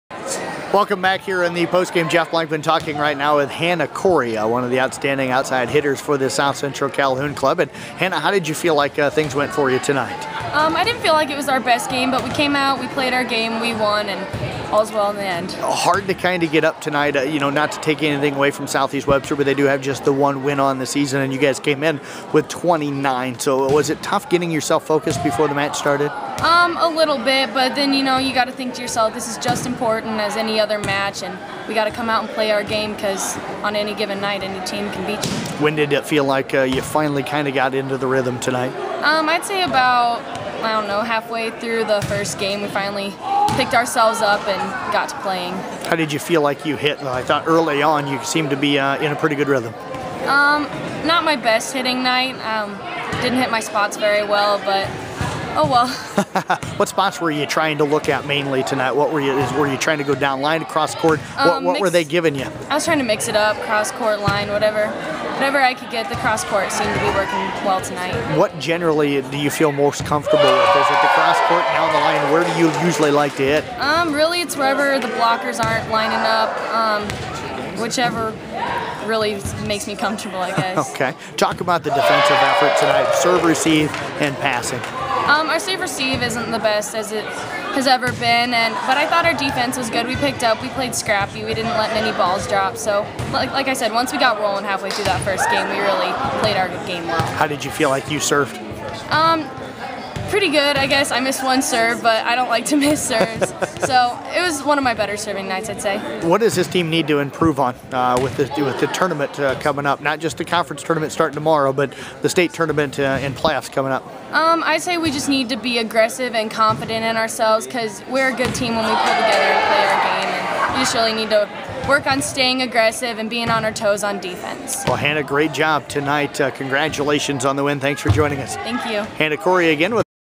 Hannah Corey of South Central Calhoun Volleyball
Corey had 5 Kills in 3-0 win over SEW